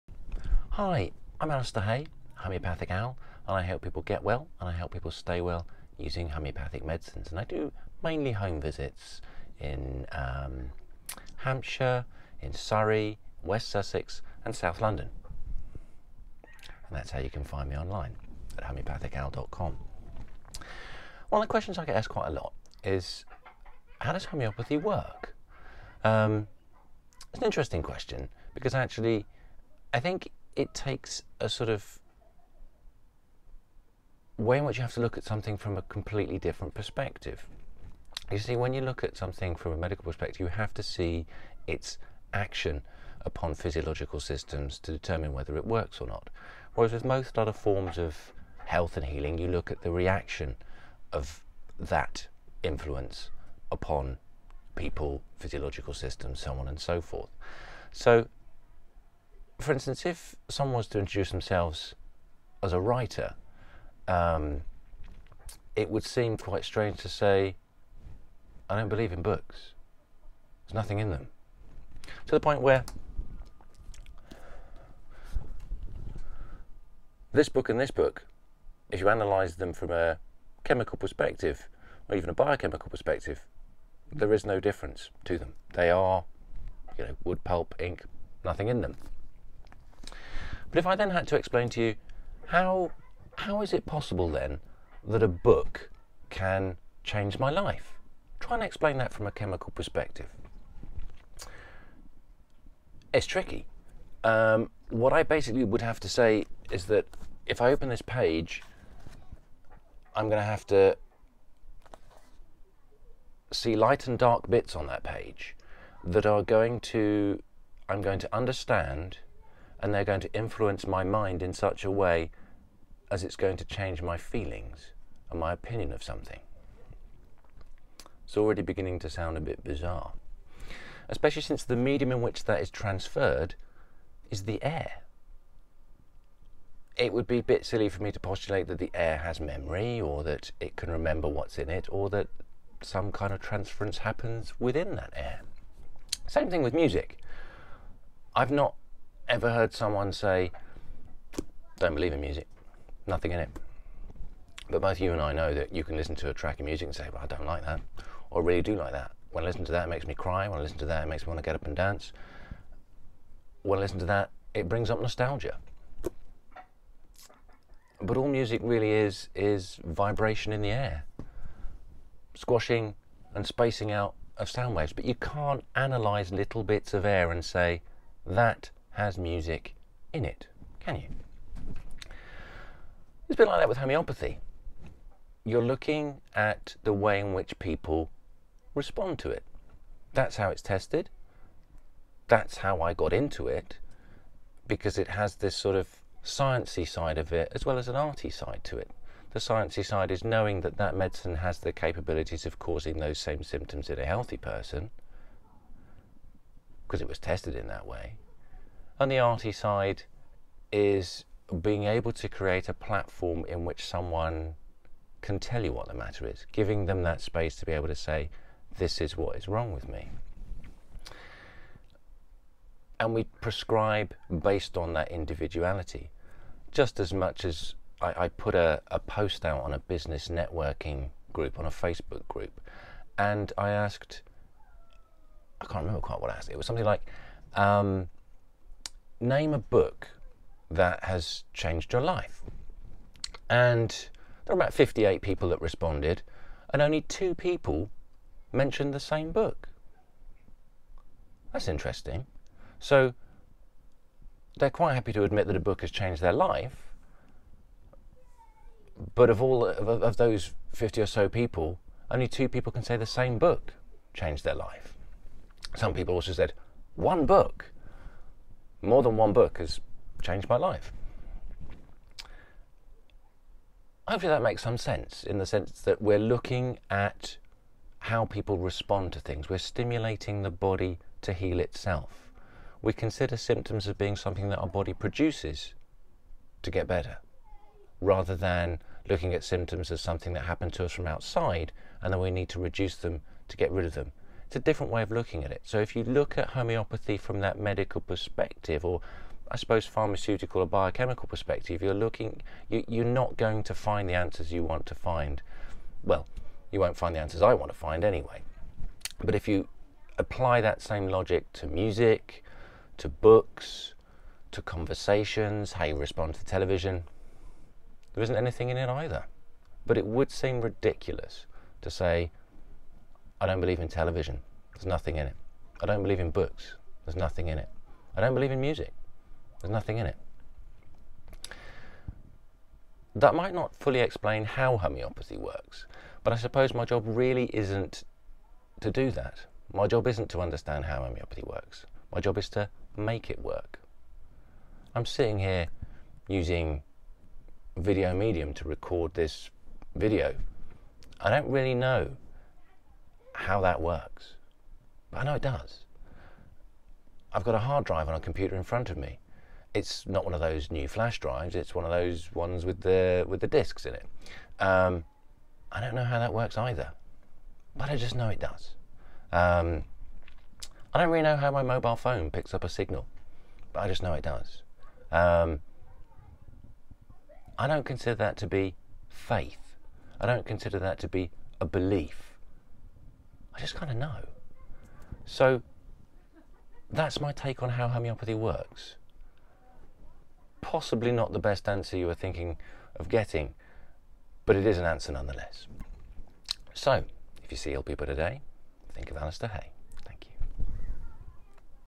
homeopathical - How does homeopathy work?
Homeopath, Alastair Hay, 'homeopathical' describes his take on how homeopathy works.